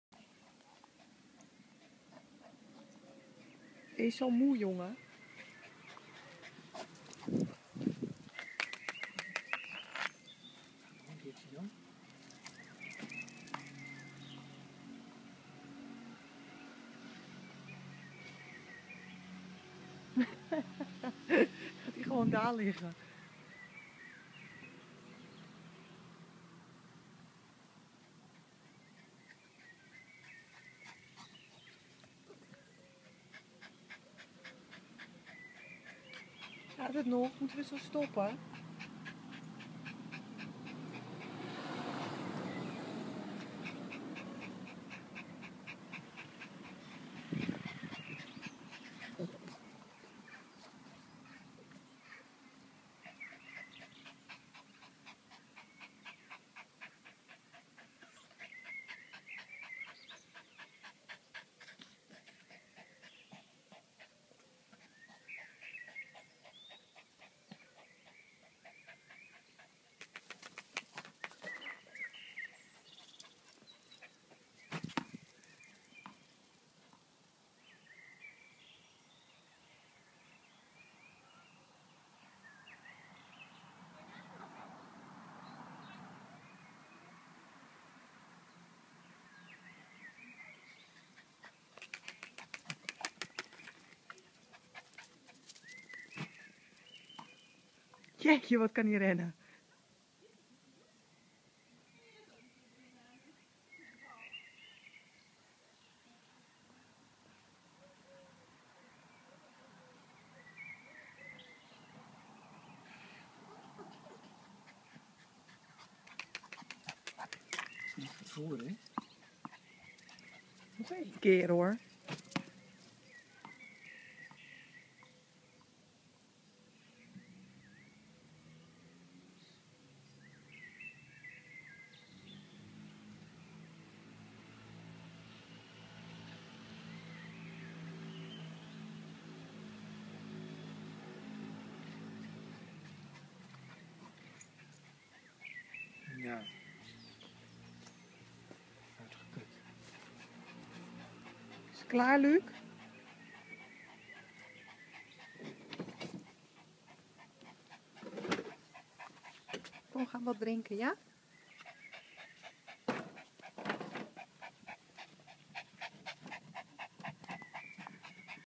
Luuk te moe om te spelen
Balspel te heftig voor kleine poedel